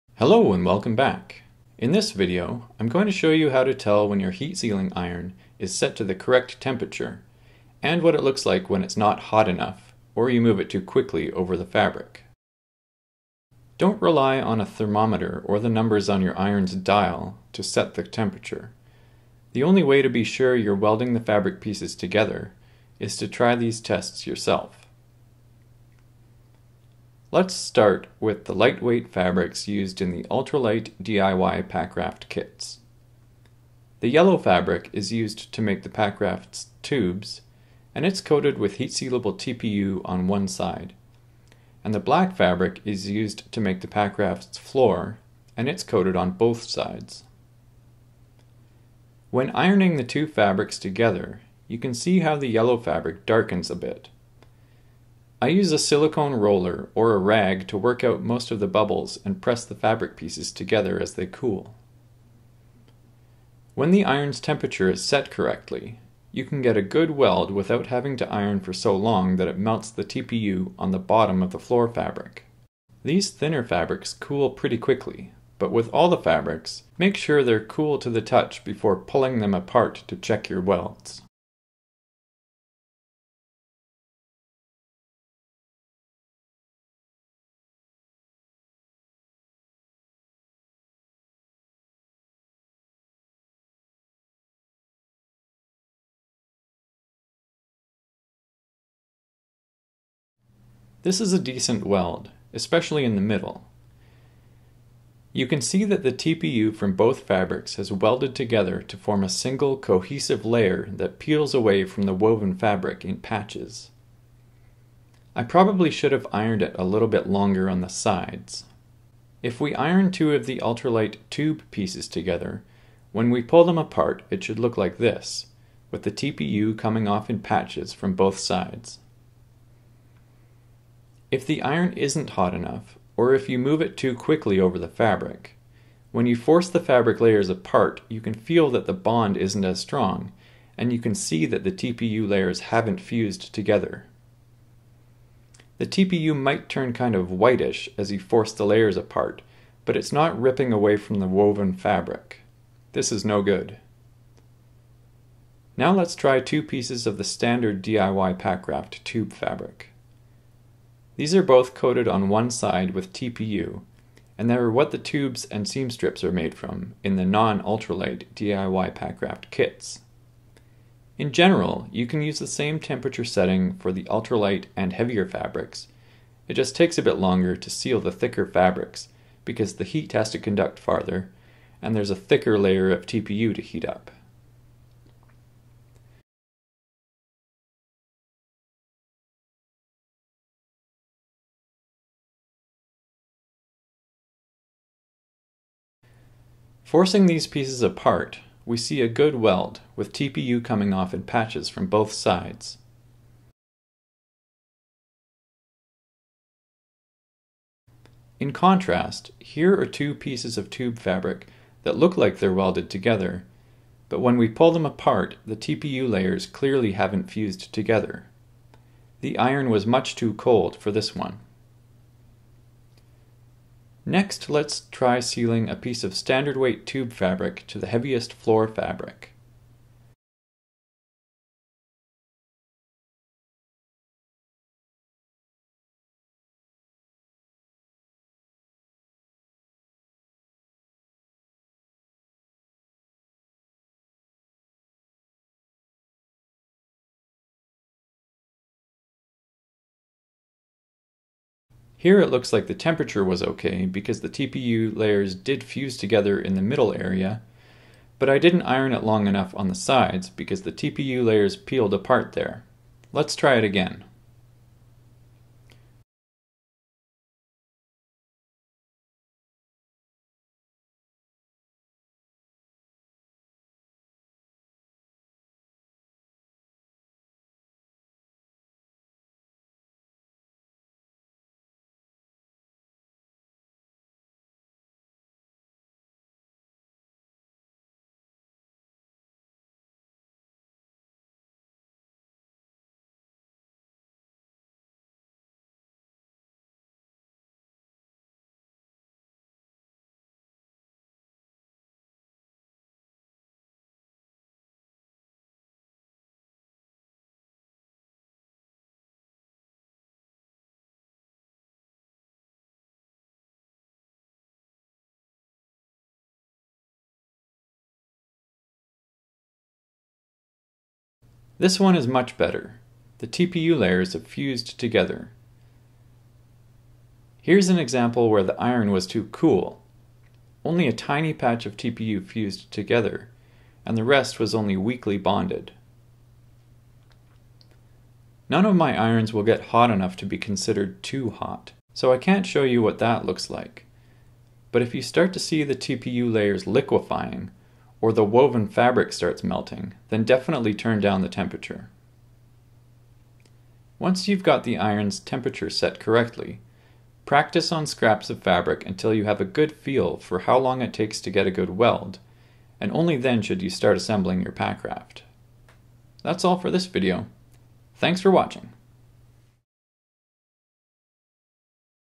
Heat Sealing Iron Temperature Setting & Timing
This video shows what to look for when setting the temperature on your heat sealing iron and how to judge the amount of time you need to spend ironing each area to create a strong weld between two pieces of heat sealable fabric.